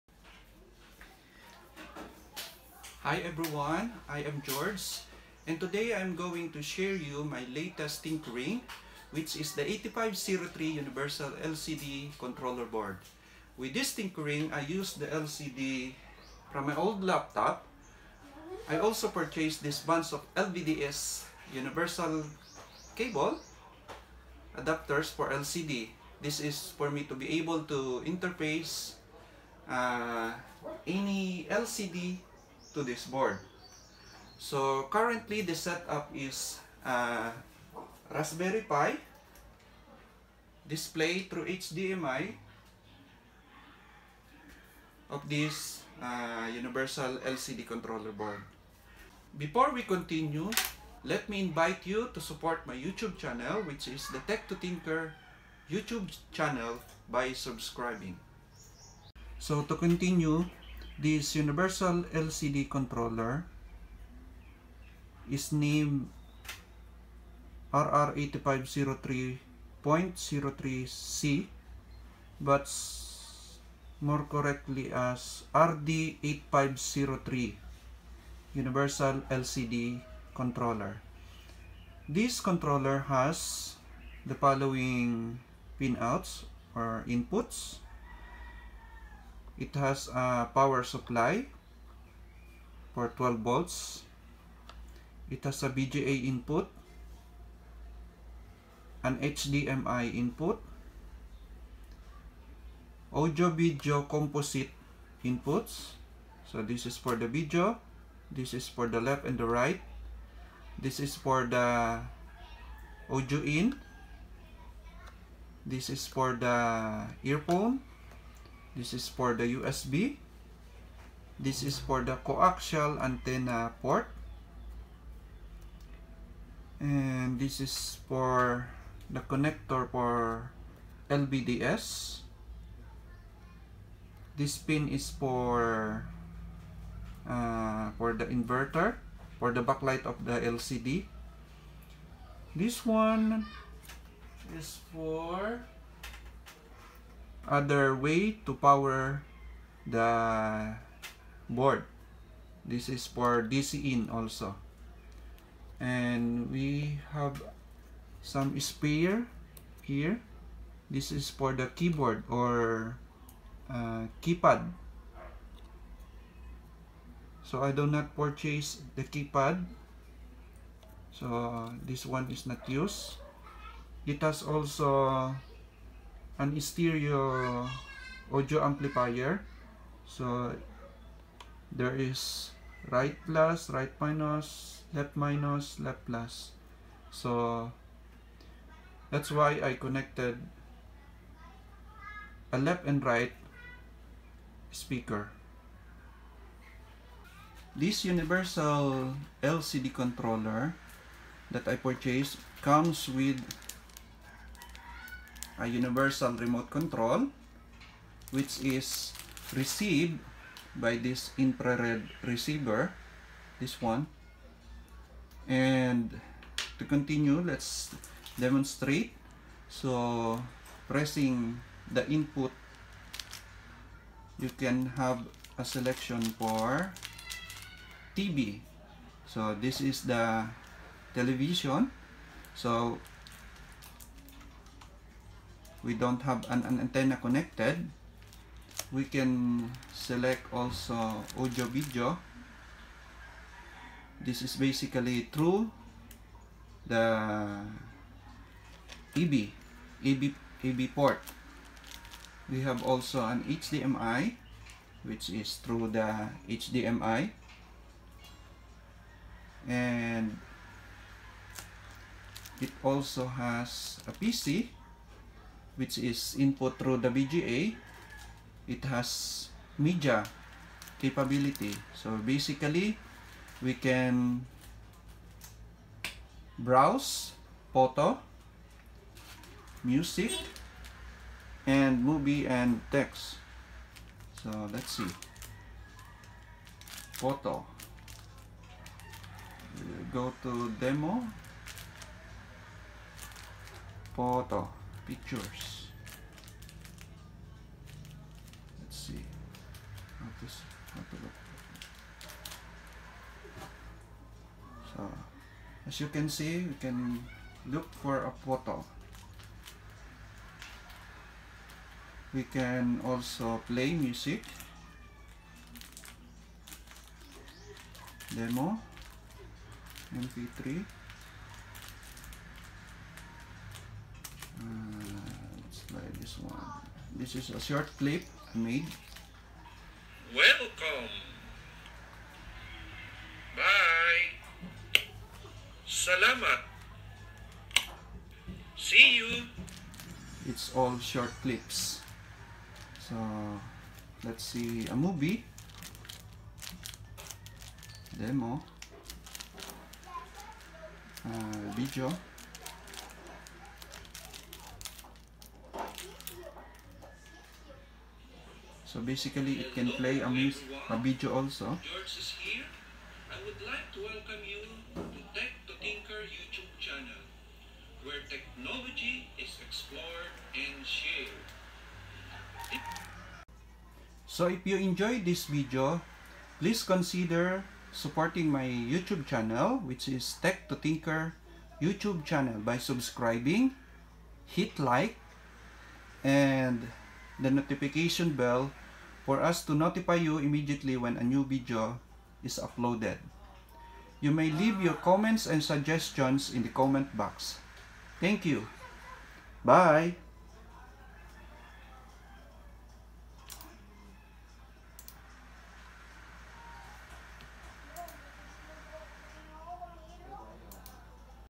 PROJECT: DIY Portable Screen Using Universal LCD Controller
TechToTinker I am wondering of what else I could do with my old broken laptop especially after harvesting its parts. The RAM is the most reusable. The hard disk is reuse by purchasing a 2.5 hdd enclosure and now functions as portable hard drive. The battery contains a couple of 8650 rechargeable batteries inside which now serves as powerbank. But when it comes to the LCD, I don't have any idea. After a couple of searching, I saw someone using his old laptop LCD as a secondary screen. So here it is!

Happy tinkering!